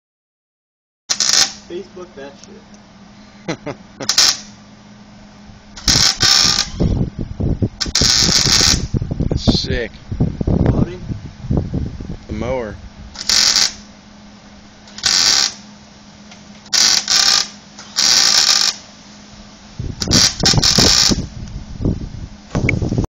Monster mower build
Doing some welding on the monster mower.